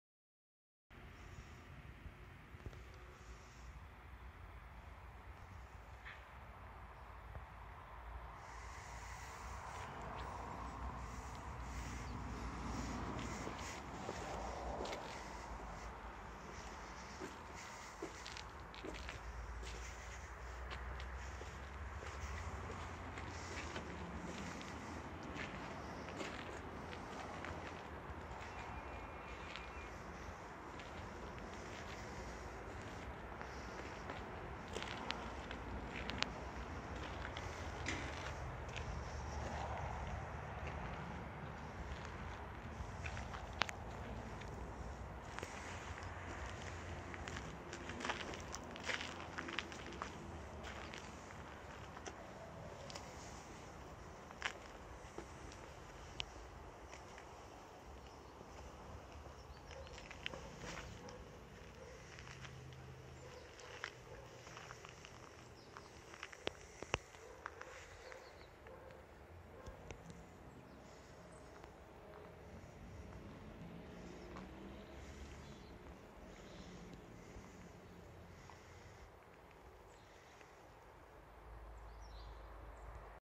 UKRAINE 🇺🇦
Ein Gebiet auf der rechten Seite der Zaporozh'ye zwischen Gang in ein Wohngegend!!!